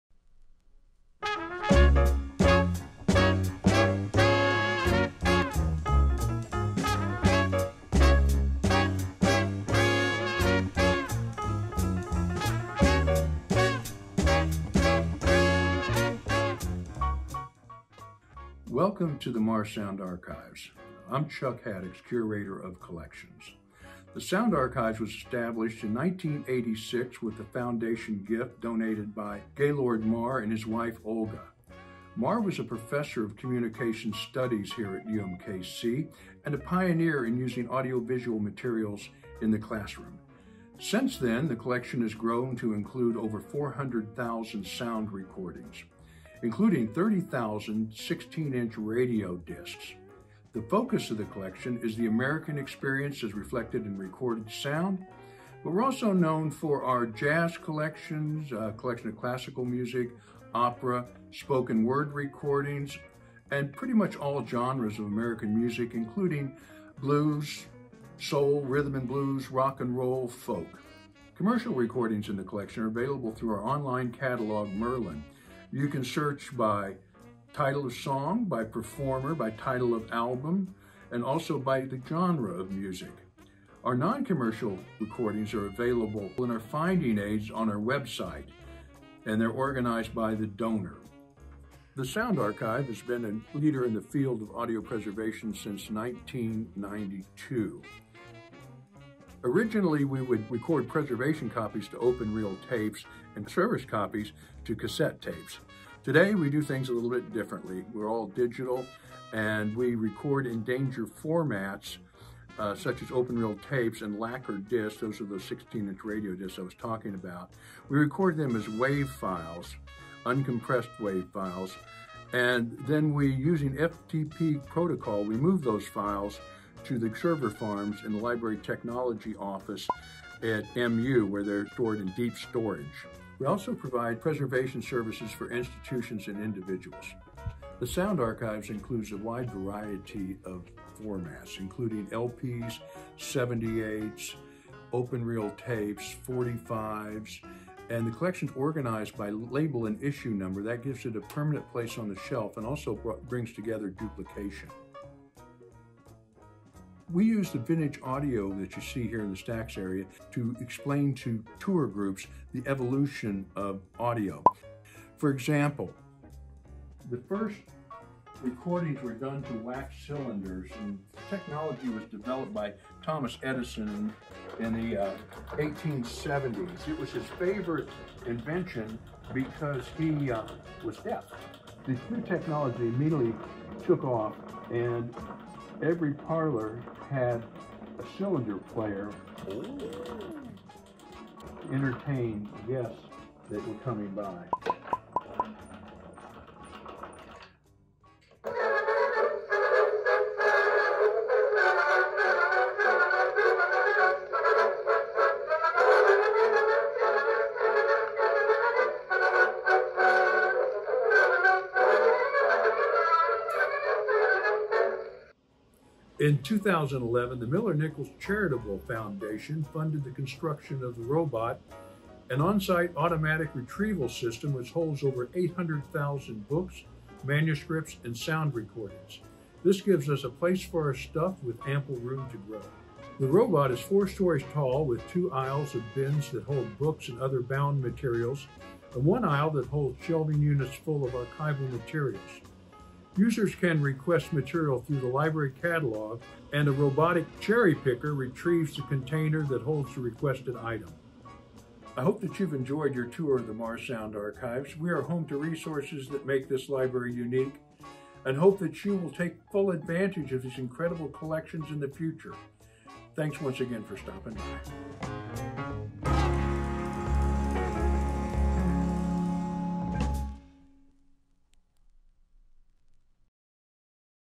Marr Sound Archives Tour
Get an inside look at the Marr Sound Archives facilities with a tour that includes an overview of the collection and a brief history of recorded sound, as well as a demonstration of legacy audio equipment.